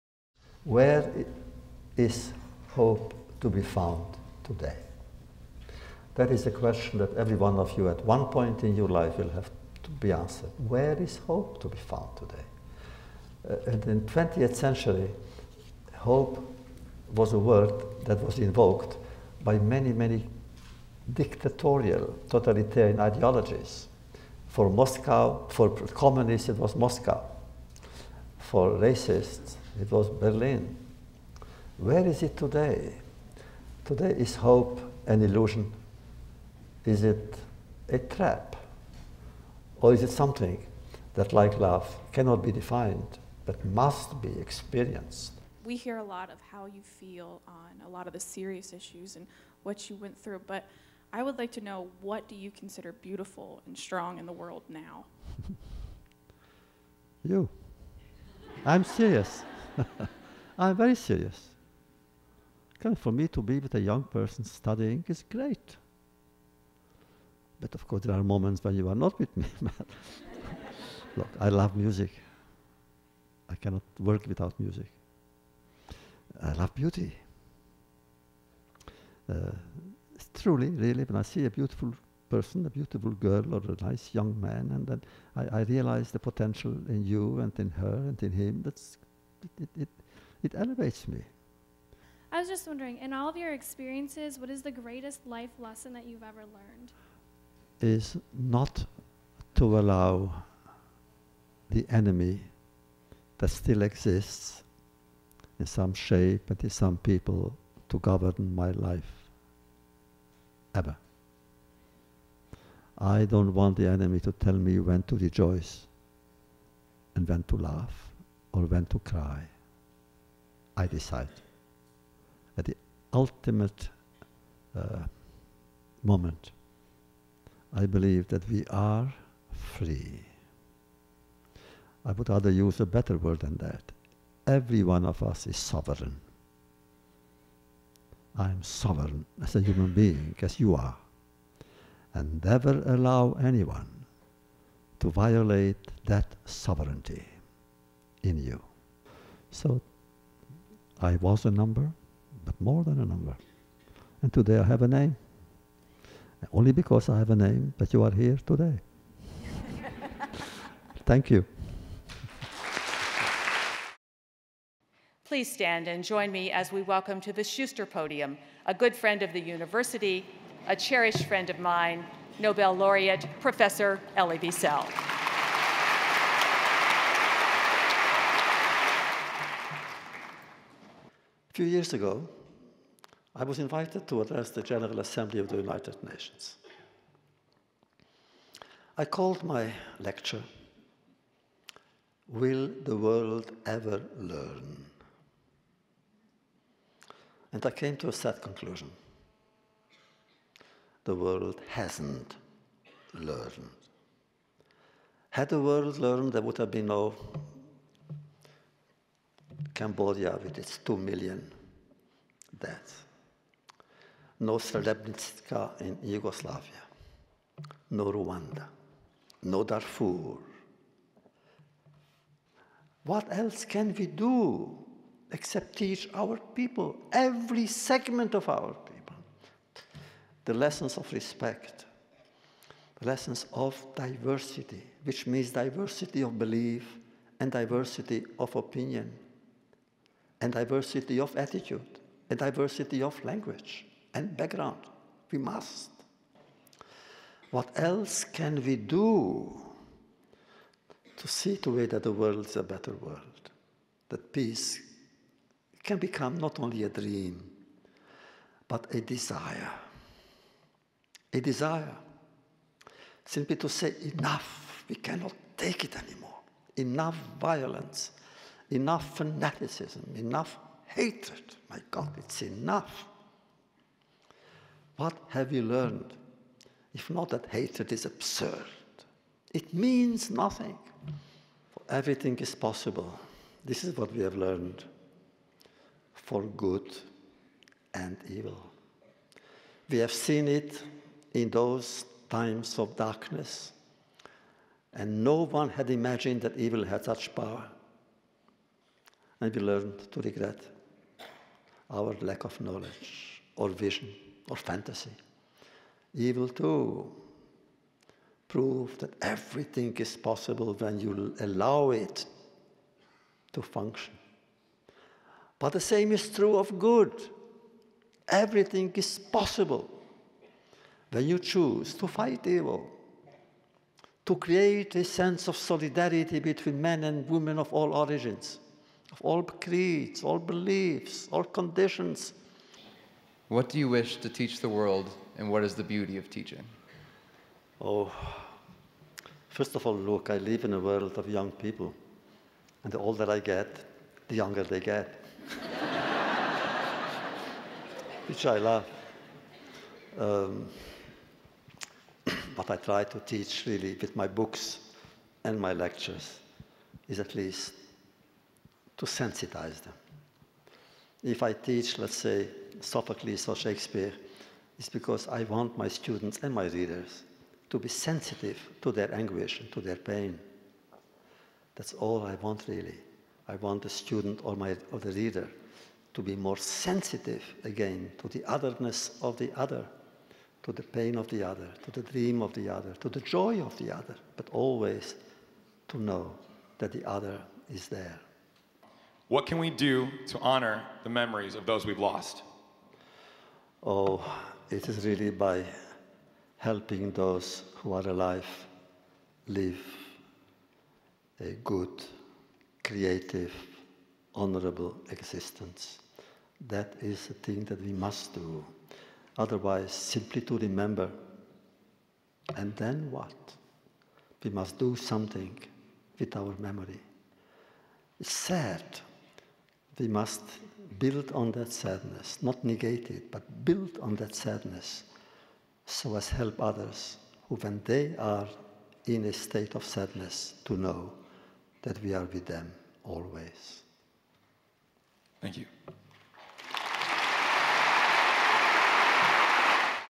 Elie Wiesel Speaks at the University of Dayton
Elie Wiesel, Holocaust survivor, prolific author and 1986 winner of the Nobel Peace Prize, rounded out the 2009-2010 Diversity Lecture Series season at the University of Dayton with a talk at 7:30 p.m. on Thursday, March 25, 2010 at the Benjamin and Marian Schuster Performing Arts Center. Elie Wiesel's appearance is presented by the University of Dayton and Victoria Theatre Association.